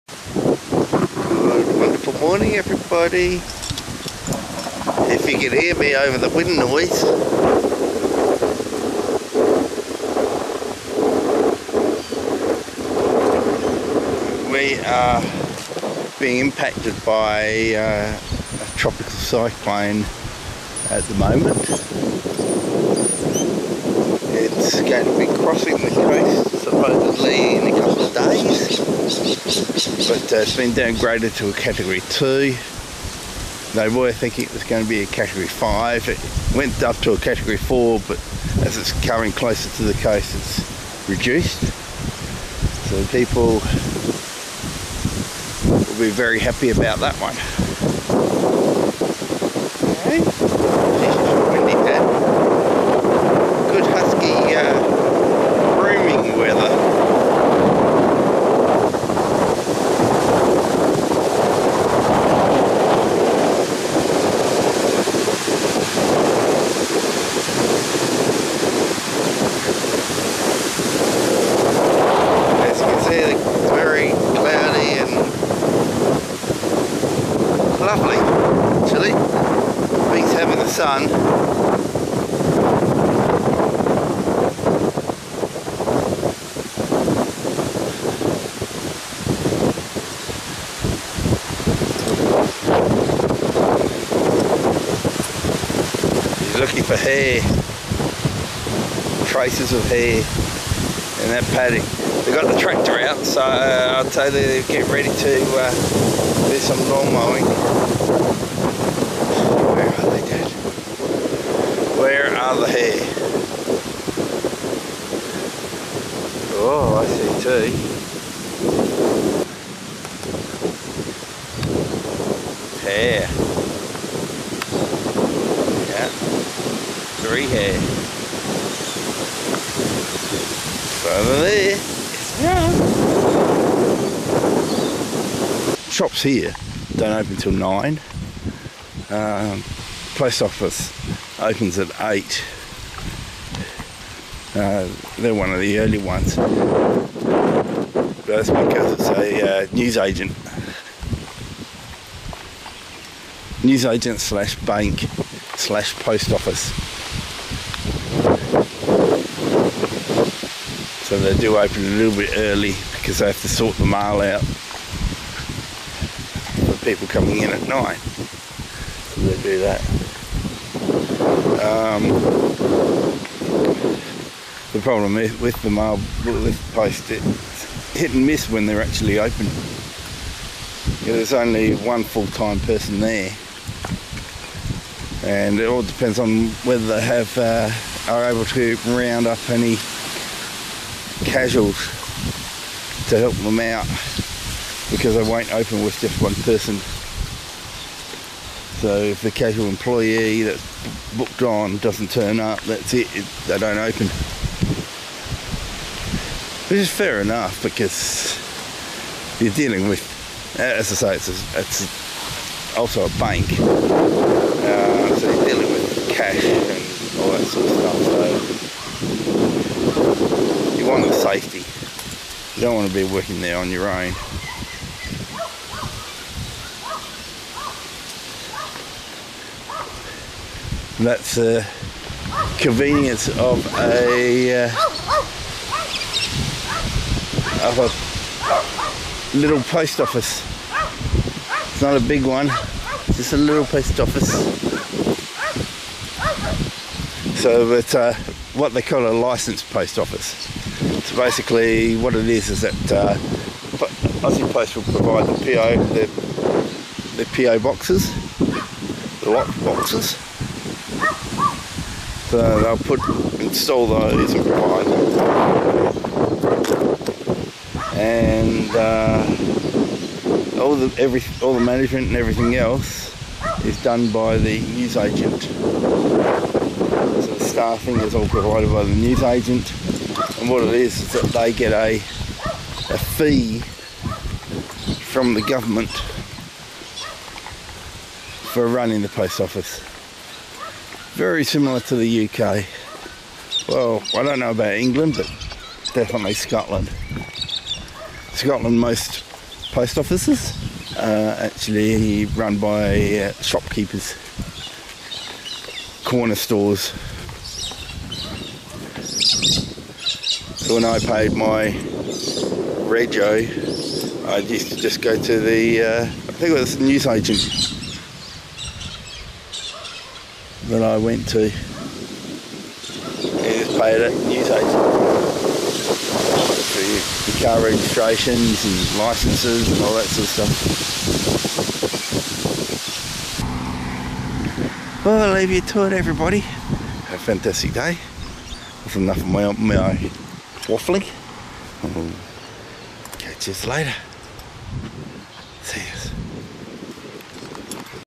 It's so windy & talking about the Post Office
We are catching the edge of the cyclone. No rain expected for us just a little wind.